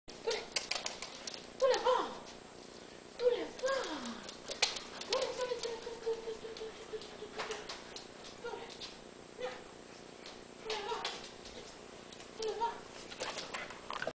Giles 11weeks a2
GILES (Spice Twice Guillemot) 11 weeks old  is looking for his own family.